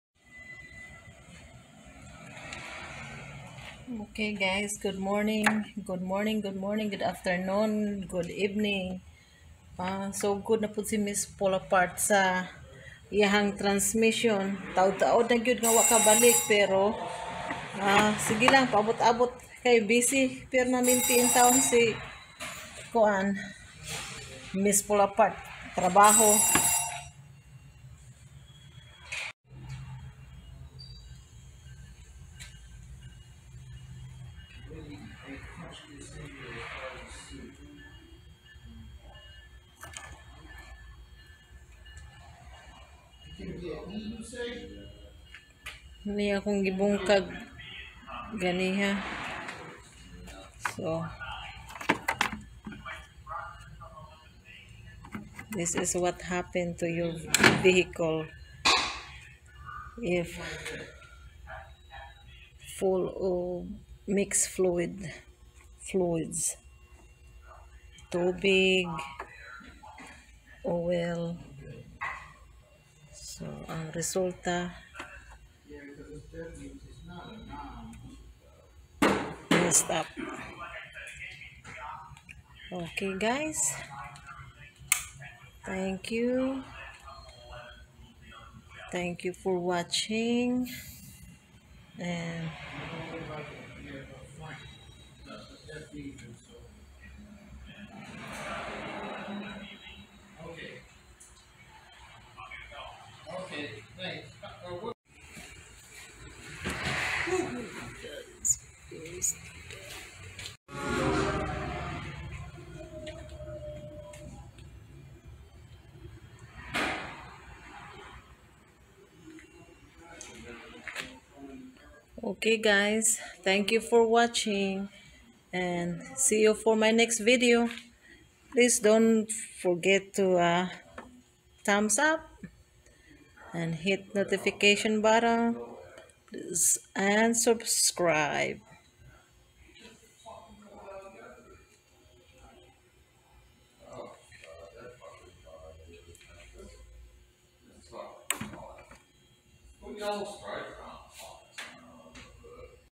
PILIPINA BACK TO HER DISMANTLE TRANSMISSION
Pilipina show what happened inside of the transmision of customer vehicle,